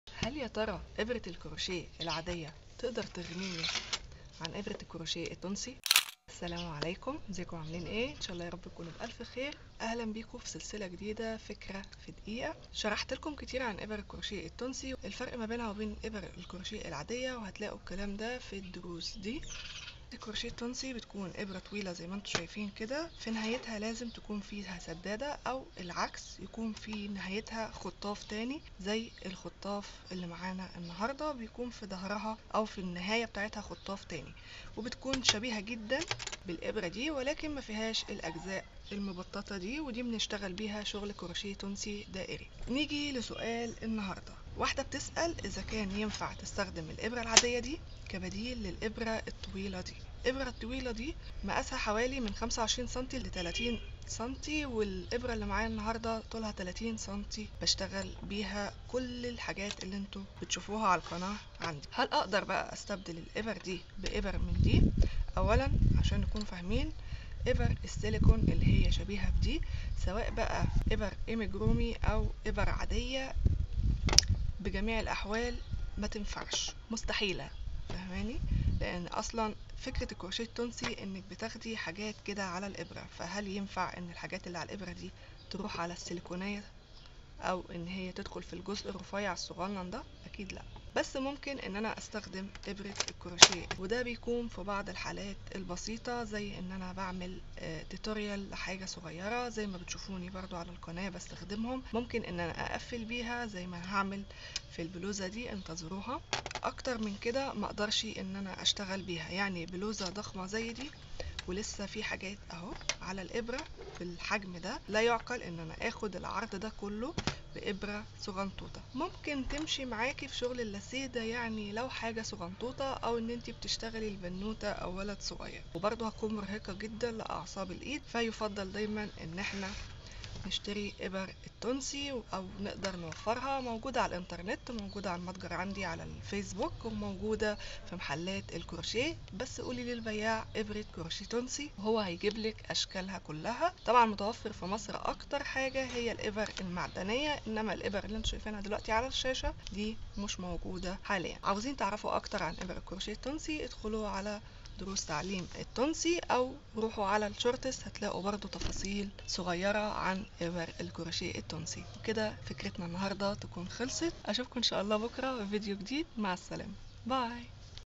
الفكرة التانية / هل ممكن استخدم الإبرة العادية بدل ابرة التونسي / crystal
و يا هلا
زي ما قلنا في الفيديو السابق .. ان افكارنا مش حتماً تكون دقيقة بالضبط ولكن هتجاوب علي اسئلتكم
و النهاردة هجاوب علي سؤال دايما بتسألوني عليه هل ممكن استبدل الإبرة التونسي بابرة عادية وامتي باستخدم كل واحة فيهم اعرفوا في الفيديو دا

او من هنا
دبابيس علامات
حبال مكرمية قطن